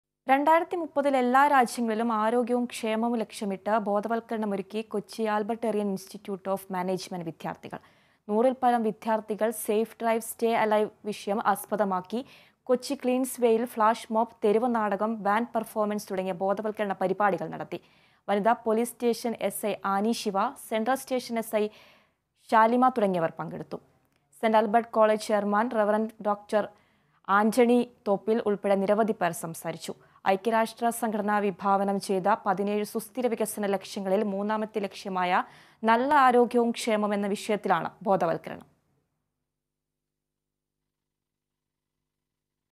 2030 ല്‍ എല്ലാ രാജ്യങ്ങളിലും ആരോഗ്യവും ക്ഷേമവും ലക്ഷ്യമിട്ട് ഒരുകൂട്ടം വിദ്യാര്‍ത്ഥികള്‍
2030 ല്‍ എല്ലാ രാജ്യങ്ങളിലും ആരോഗ്യവും ക്ഷേമവും ലക്ഷ്യമിട്ട് ബോധവല്‍ക്കരണം ഒരുക്കി കൊച്ചി ആല്‍ബര്‍ട്ടിയന്‍ ഇന്‍സ്റ്റിറ്റ്യൂട്ട് ഓഫ് മാനേജ്‌മെന്റ് വിദ്ധ്യാര്‍ത്ഥികള്‍.

About COA